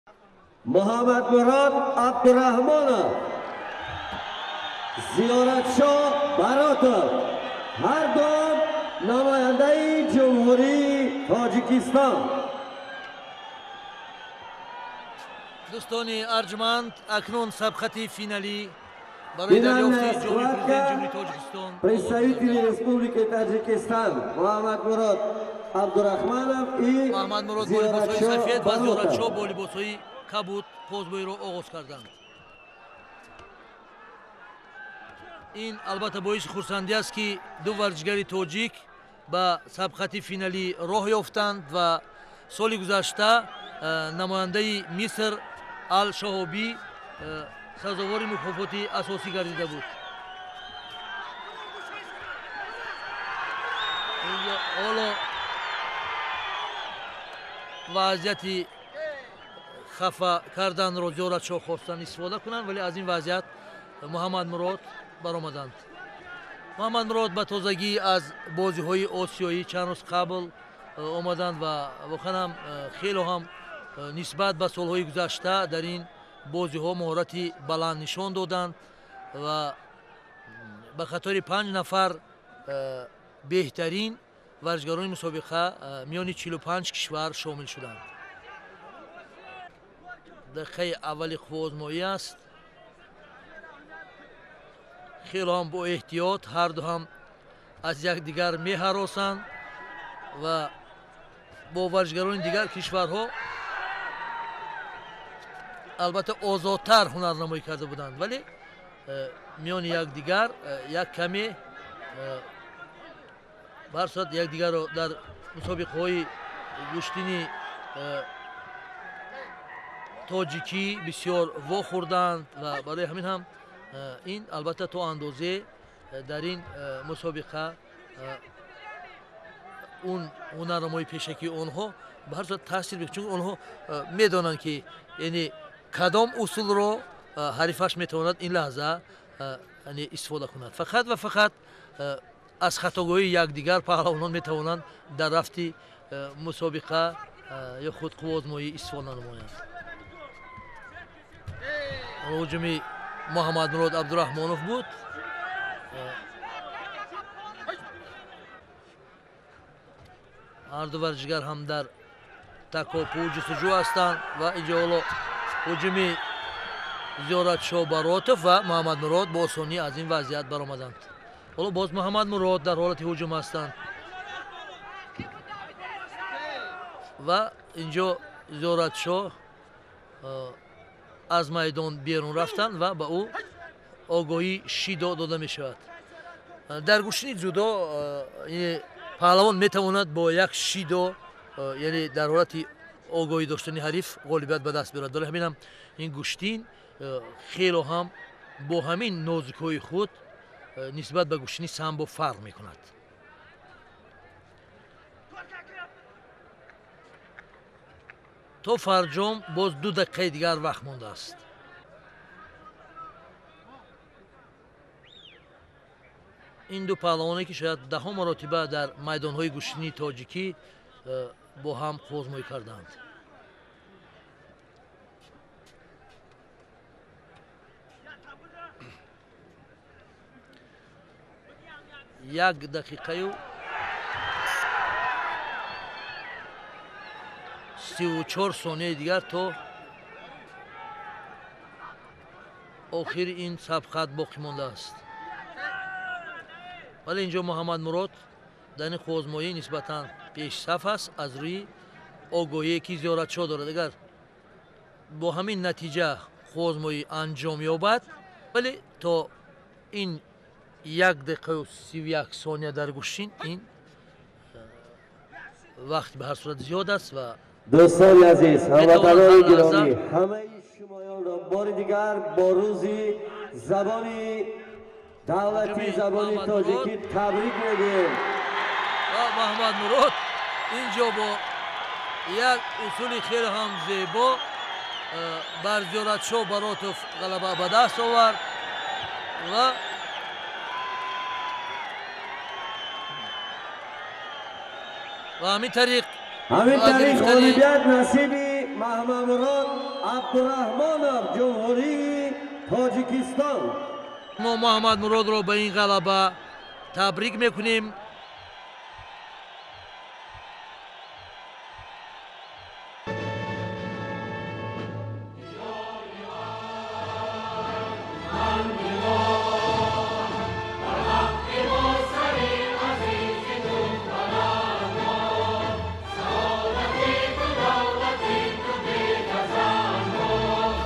Аз бойгонӣ. Муҳаммадмурод (Ҷага) vs Зиёратшо Баротов дар финали Ҷоми Президенти ҶТ оид ба ҷудо-2014
Аз бойгони / Мухаммадмурод Абдурахмонов / Чага / Muhammadmurod Abdurahmonov / Jaga Judo / Чоми Президенти Точикистон оид ба чудо /